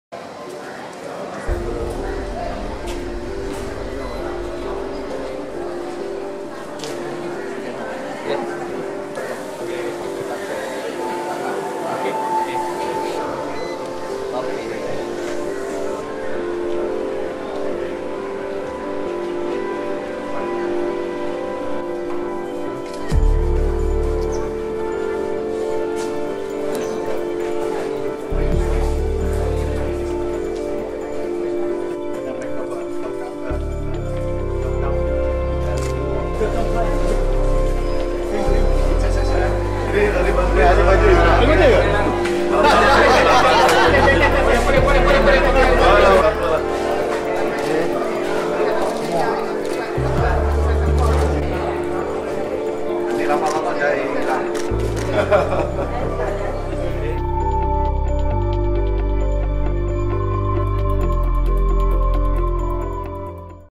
威省市政厅 启动初步防疫 公众必须接受体温检测
（大山脚11日讯）威省市政厅即日起启动武汉肺炎（2019冠状病毒疾病）初步防疫措施，所有进入市厅大厦办事的公众须经过体温检测，柜台服务员戴手套和口罩办公。

　市厅也议决延后参与国际性活动或取消远途出差；至于市厅在本地举办的活动，还是如期展开，但以小规模及做好防疫措施的情况举行。

拍摄：罗健杰